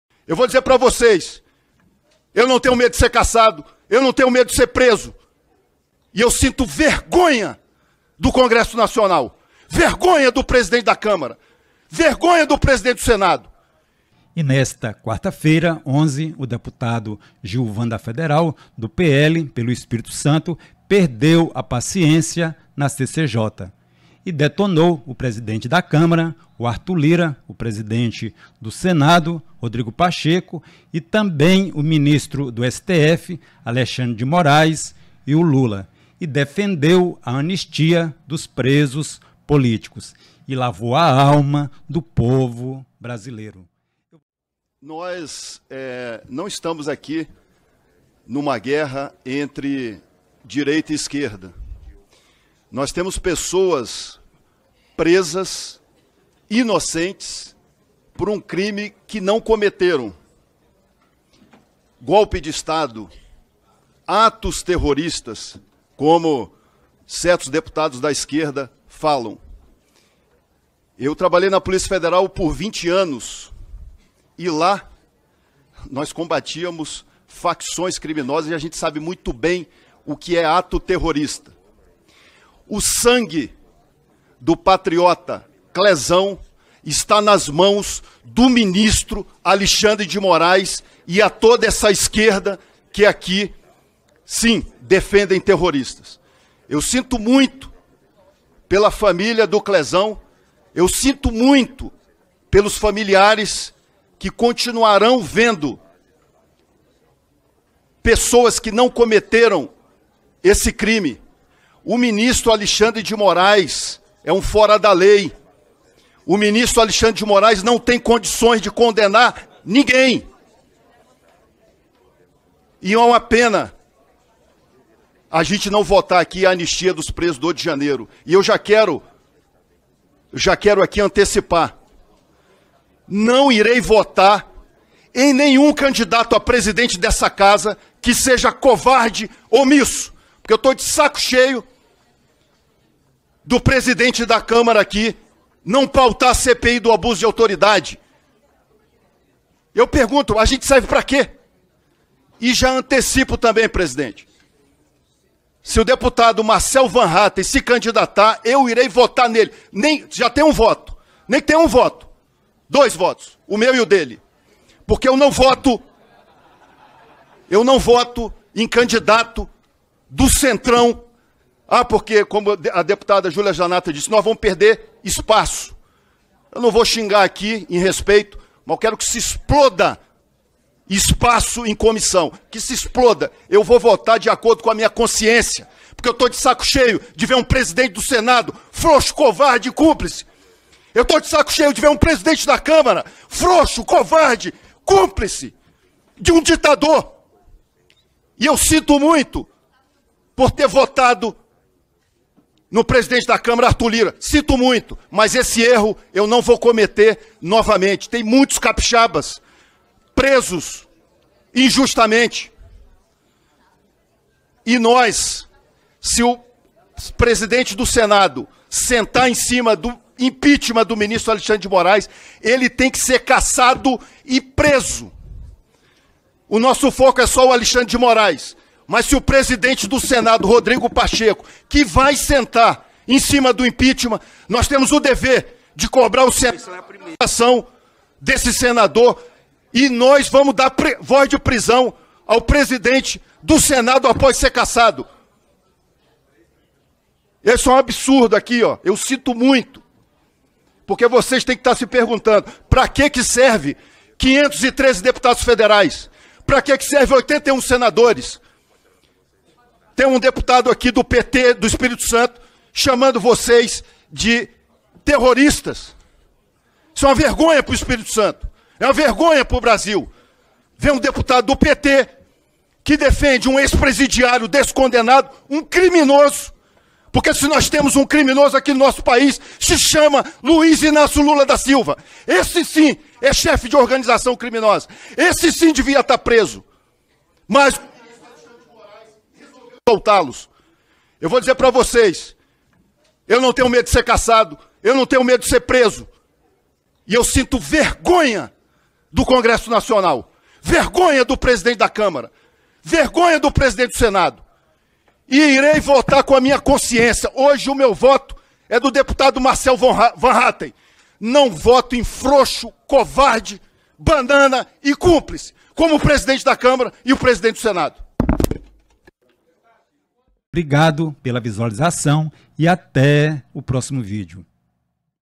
DEPUTADO LAVA A ALMA DO POVO BRASILEIRO
Deputado federal Gilvan da federal (PL - ES), perdeu a paciência na CCJ nesta quanta-feira 11.